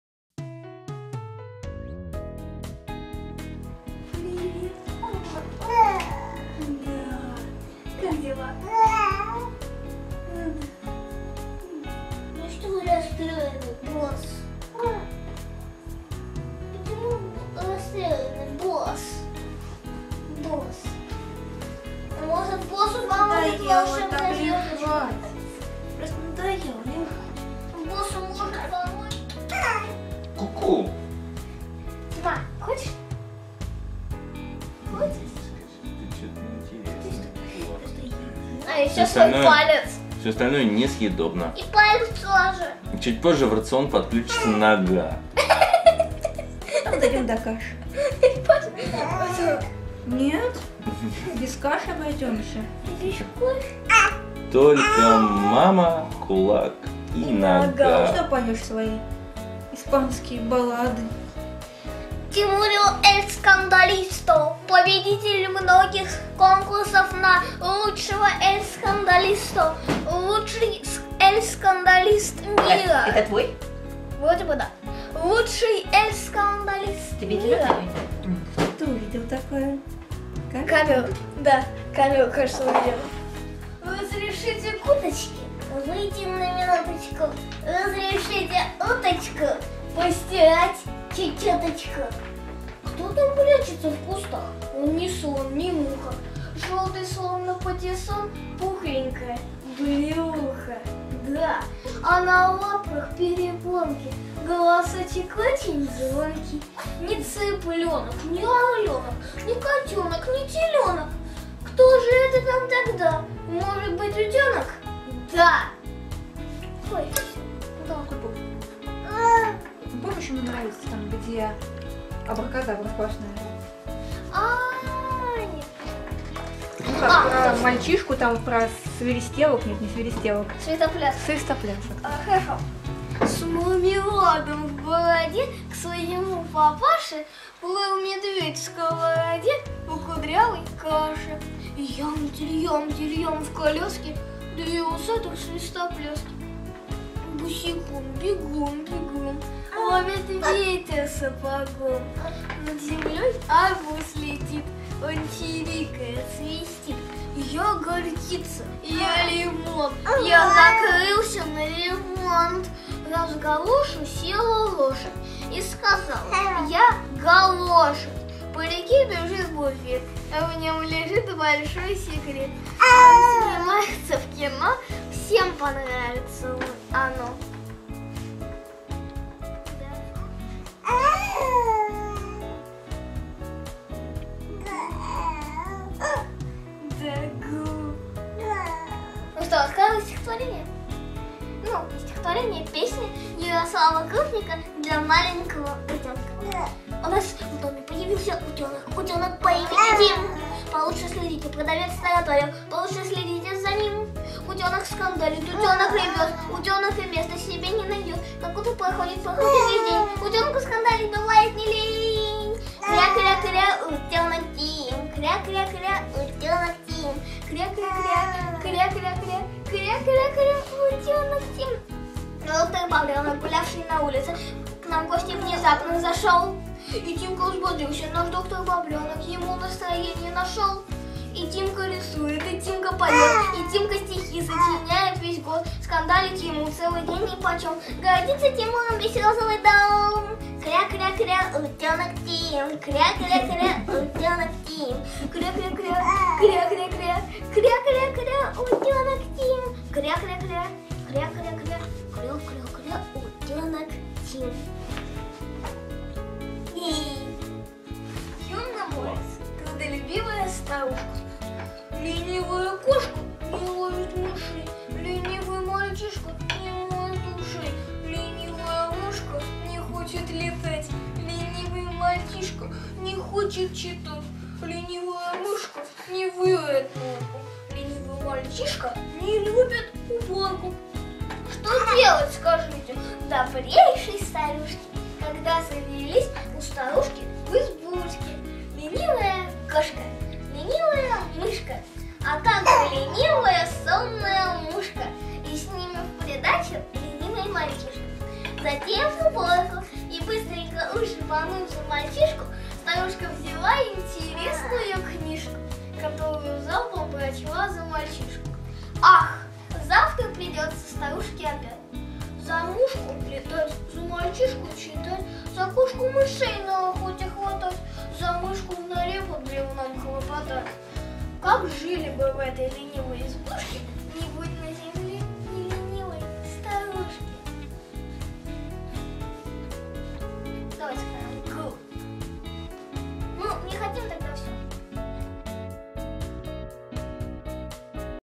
Яр и Тим
Стихи и песни или созданы Ярославом Крупником, или взяты из учебника по литературному чтению. Внимание. Яр и Тим поют куплеты!